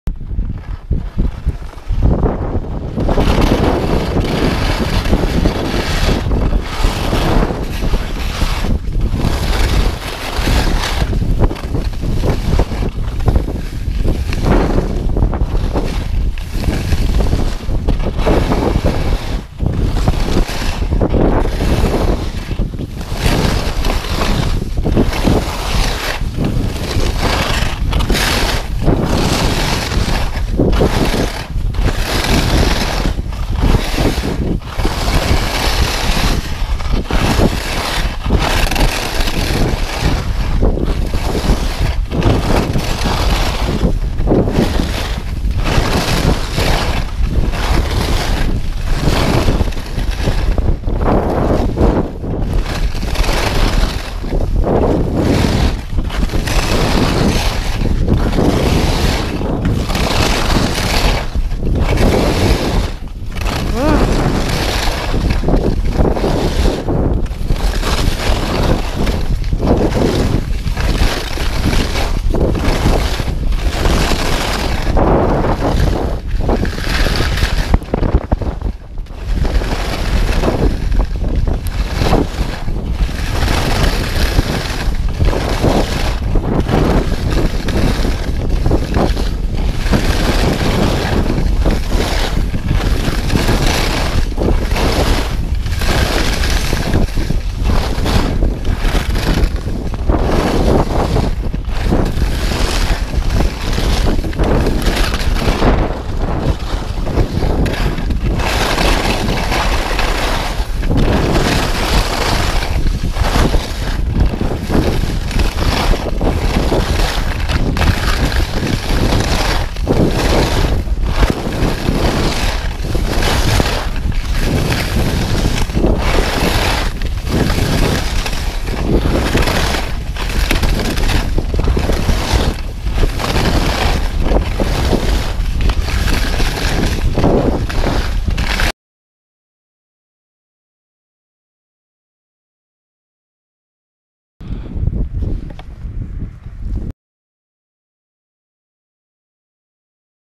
Sugarloaf-Gondi Line
Re filmed in the snow. A black run on the upper mountain.